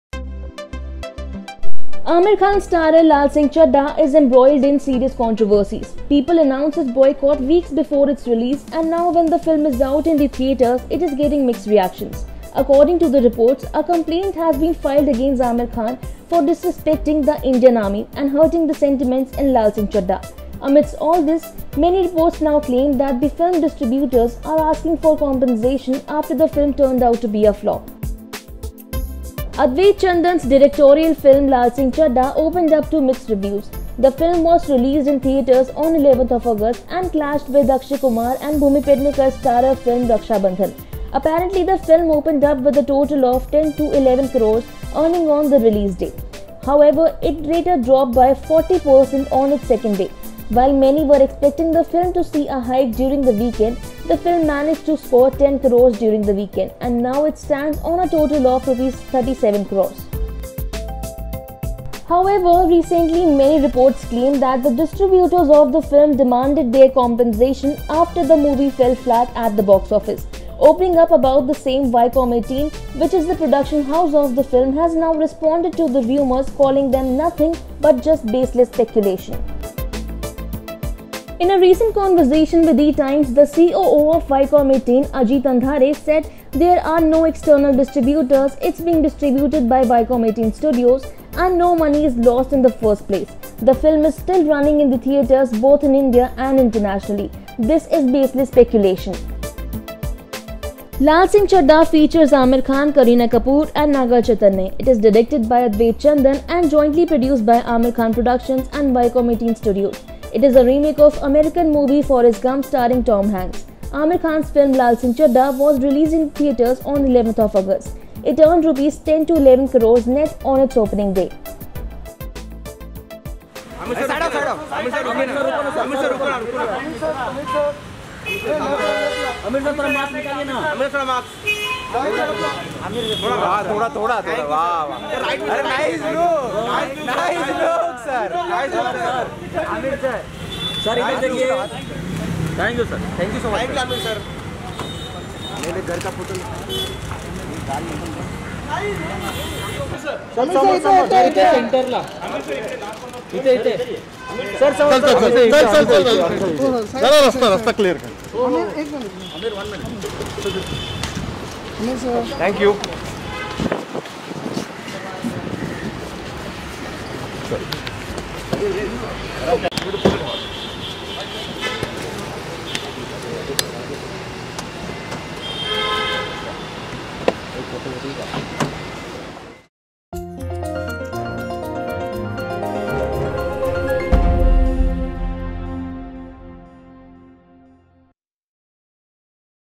Laal Singh Chaddha Makers Opens Up On Rumors About Distributors Asking For Compensation
Aamir Khan’s Laal Singh Chaddha is embroiled in serious controversies. People announced its boycott weeks before its release and now when the film is out in theatres, it is getting mixed reactions. According to the reports, a complaint has also been filed against Aamir Khan for “disrespecting the Indian Army” and “hurting sentiments” in Laal Singh Chaddha. Amidst all this, many reports now claim that the film distributors are asking for compensation after the film turned out to be a flop.